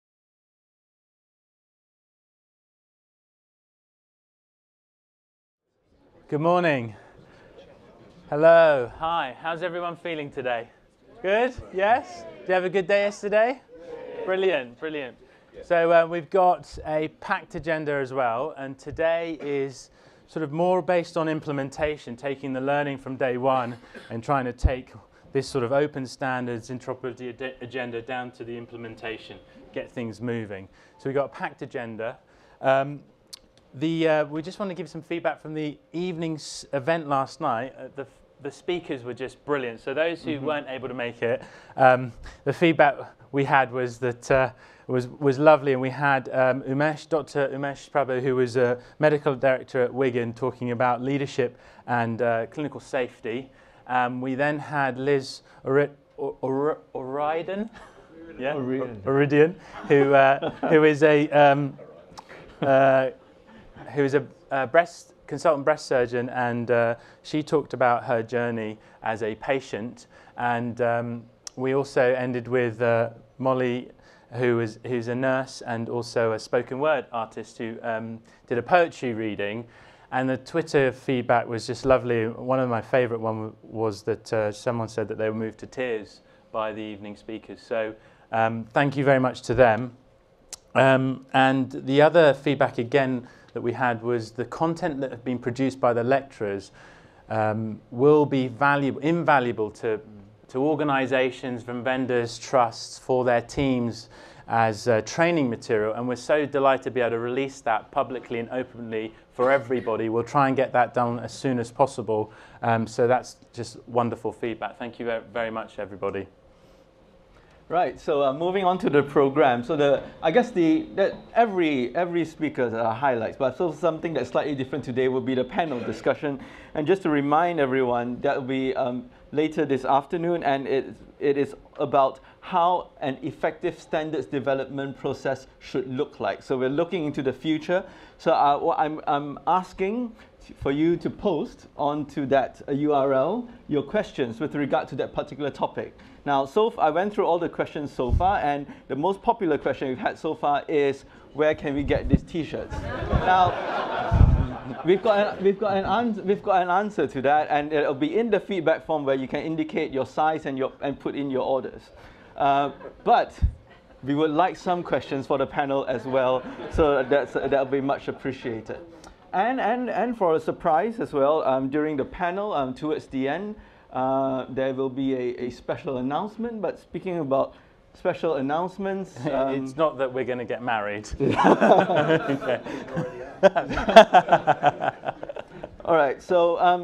Day 2-01 Opening
Opening Statements on Day 2 by Co-founders Wai Keong Wong (@wai2k) and Amir Mehrkar (@DrAmirMehrkar)